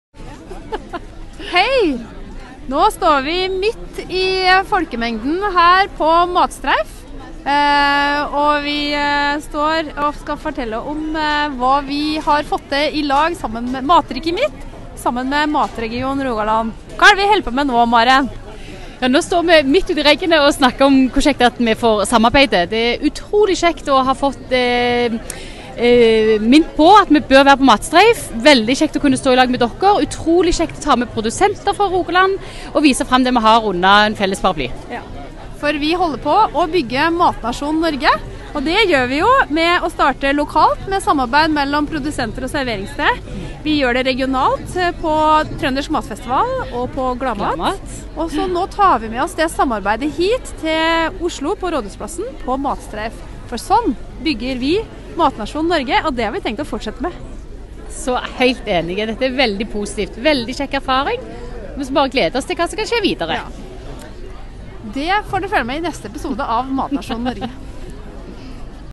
Trøndelag og Rogaland sammen på Matstreif 2019
Mat- og landbruksdepartementet ønsker å bygge matnasjonen Norge. Da trengs det sterke matregioner, mener aktører i Trøndelag og Rogaland. De sto derfor sammen om å vise fram lokale mat- og drikkespesialiteter under Matstreif 2019.